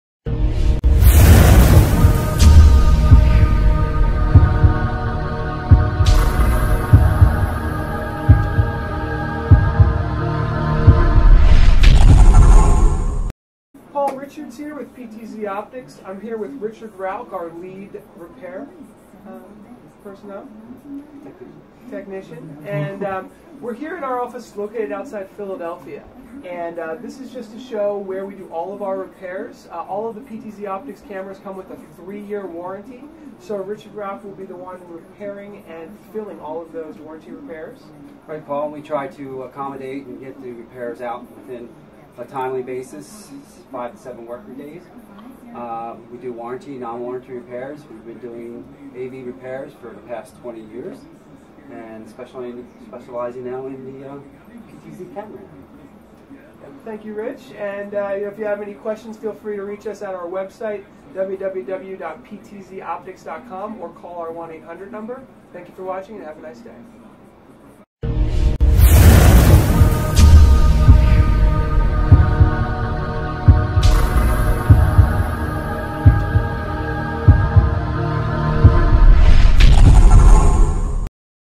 PTZOptics USA Repair Center
A quick look into our PTZOptics USA Repair Center which is 100% solar powered! This is where your PTZOptics warranty repair will be facilitated here in the United States.